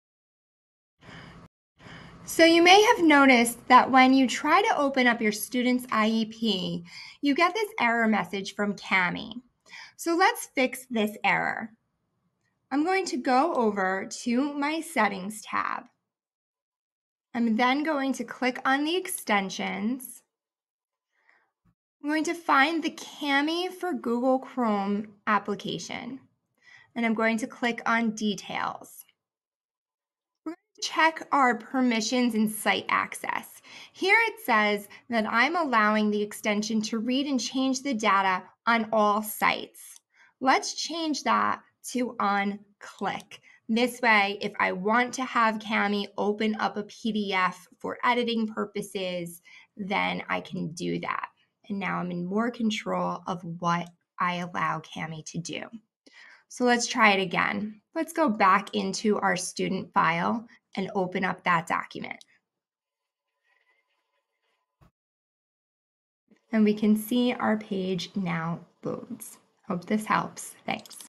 Setting Site Access Permissions in Google Chrome
Changing extension permissions so that documents successfully open.